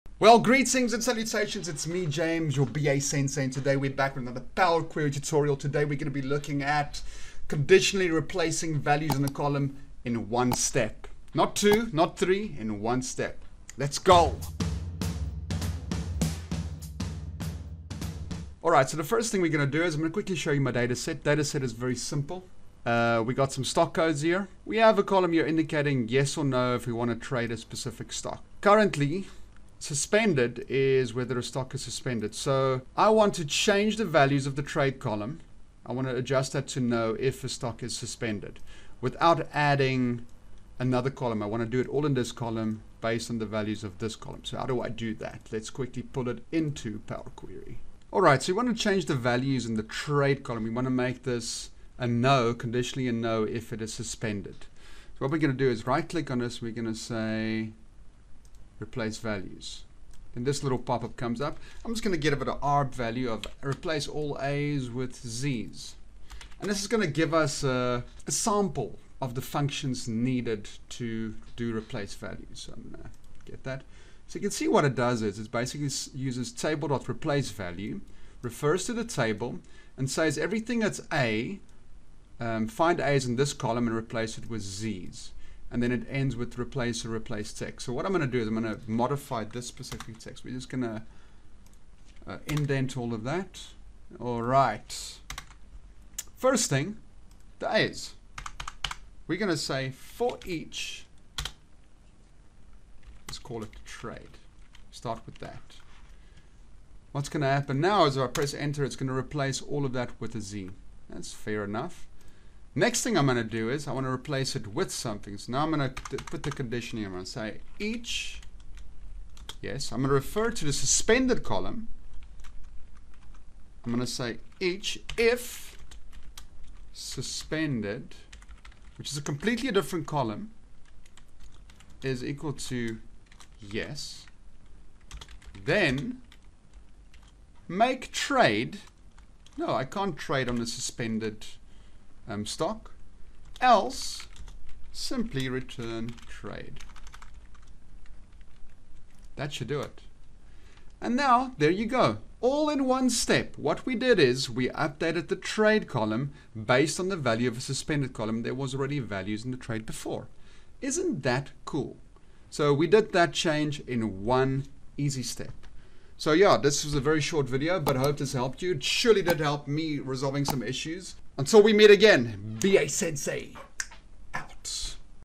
Power Query - Conditionally replace values in one step!
Replacing Values in a column is a common requirement and you'll run into it a lot in real world data. But it usually (not using advanced editor) takes couple of steps to get the values replaced. In this video i show you how to do it in one step using a bit of m-query in the advanced editor.

Links
Sample File used in Video